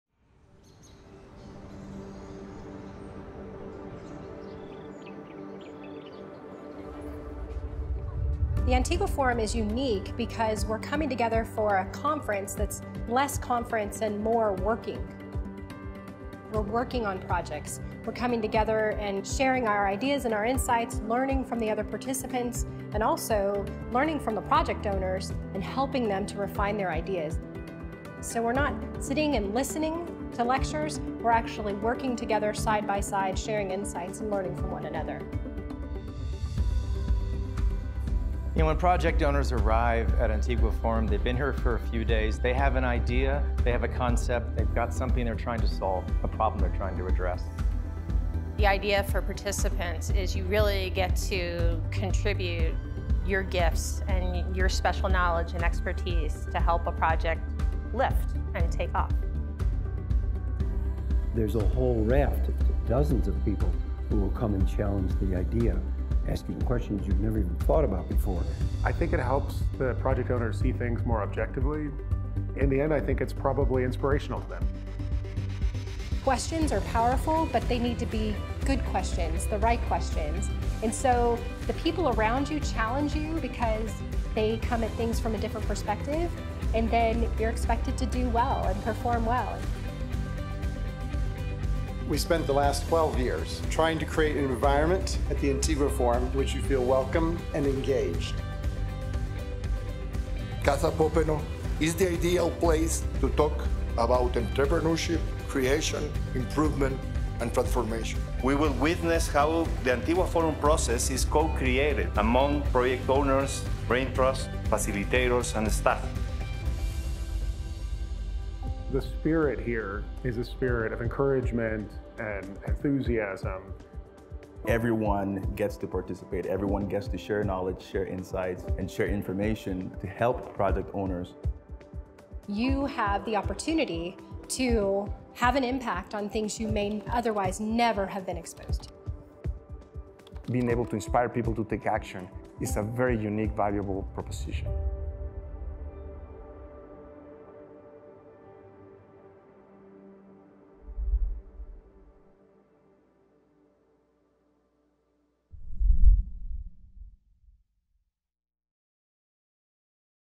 Antigua Forum 2023 | The Accelerator for Freedom
The Antigua Forum is an exclusive, annual gathering of political reformers and entrepreneurs from around the world who believe in the power of human ingenuity and free enterprise to dramatically improve the well-being of people everywhere. This event, of collaborative nature, focuses on functioning as a hands-on workshop rather than a traditional conference. Participants engage in refining and developing project ideas by sharing expertise and challenging project owners with new perspectives and questions.

The forum creates an environment that encourages critical thinking, creativity, and inspiration, helping project owners see their ideas more objectively. Over the past 12 years, the forum has built an atmosphere that supports entrepreneurship, improvement, and transformation. The event allows all participants to share knowledge and inspire actions beyond the forum.

The Antigua Forum is a project of Universidad Francisco Marroquín in Guatemala.